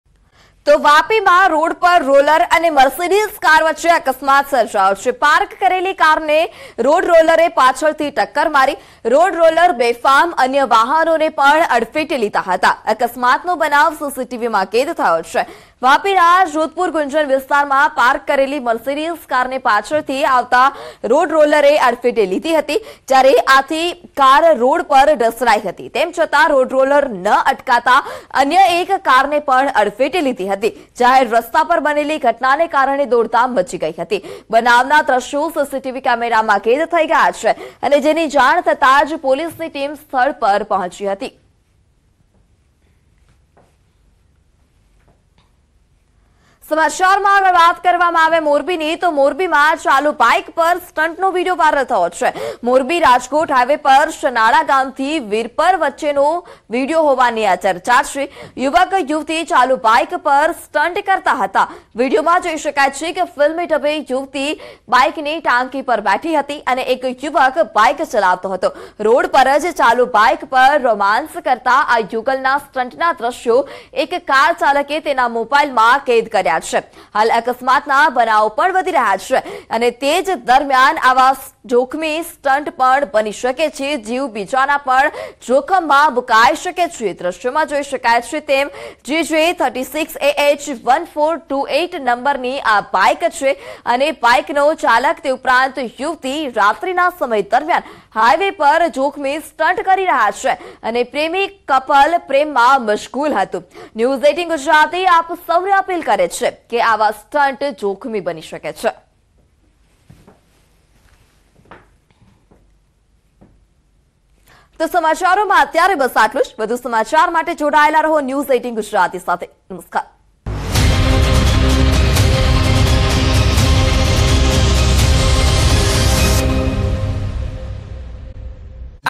Vapi News | પાર્ક કરેલી કારમાં ઘુસ્યુ રોડ રોલર | Viral Video | Accident News | Road Accident |News18
An accident between a road roller and a Mercedes car in Vapi hit the parked car

વાપીમાં રોડ રોલર અને મર્સિડીઝ કાર વચ્ચે થયો અકસ્માત, પાર્ક કરેલી કારને મારી ટક્કર.n18_south-gujarat